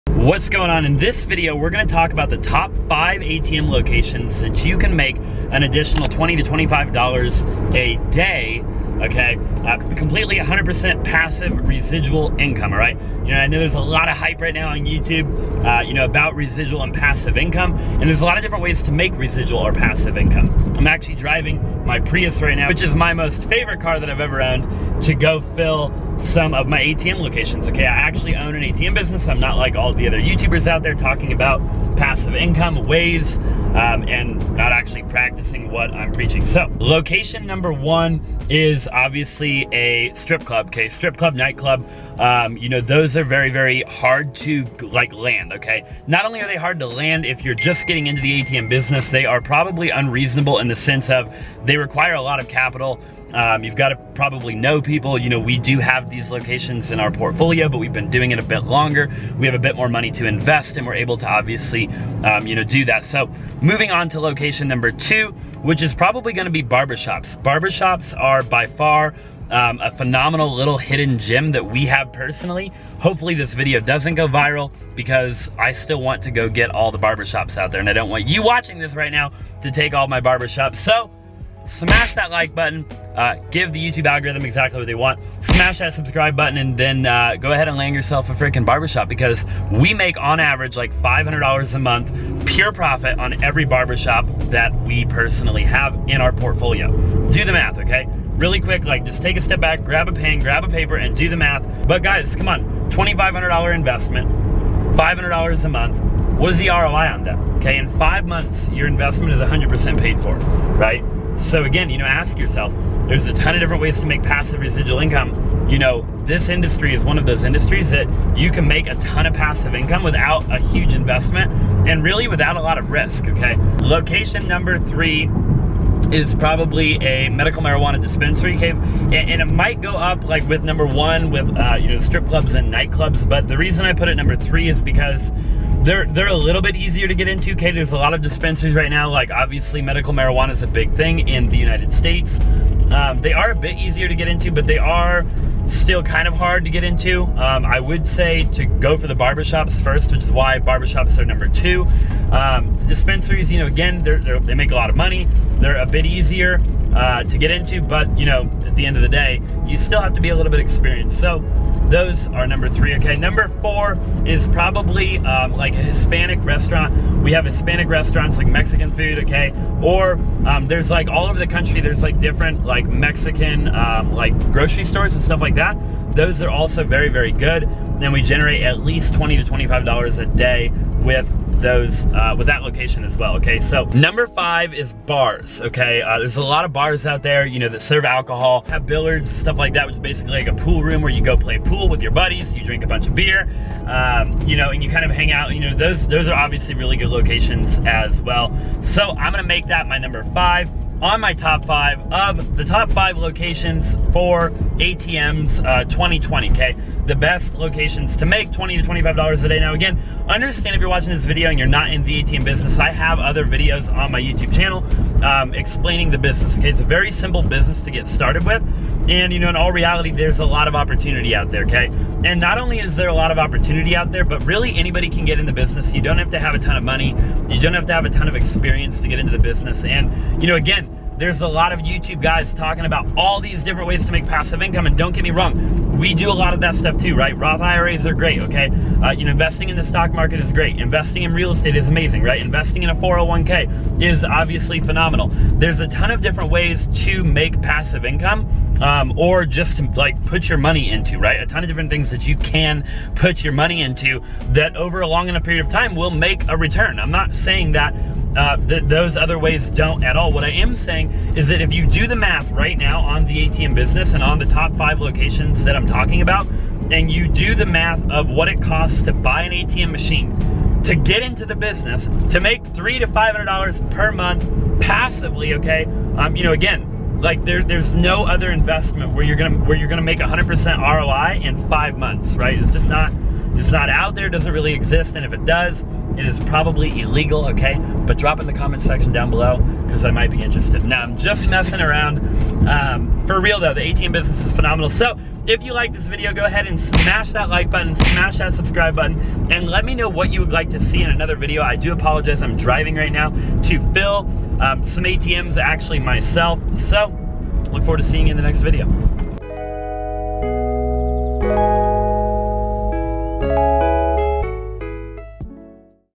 Top 5 ATM Locations | $25/Day Passive Income
Ever wondered who owns those ATM Machines everywhere? Or wanted to know what it really takes to start an ATM business? Tune in— I will cover the cost to start an ATM Business and all the main things you need to know. The investment required to start an ATM Business may seem out of your budget, but it’s not as much or as complicated as you think!

In this video, I’ll go over the best and my favorite locations to place ATMs in, the cost of an ATM Machine, the amount of cash required to put into the ATM Machine, and MORE!

The ATM Business is one of my favorite ways to earn passive income. It’s a phenomenal way to ensure you’re making residual income at all times.

💵 ATM Necessities

I truly appreciate all of your support. 🙏 Drop a comment if you have any questions or want to suggest a video idea about: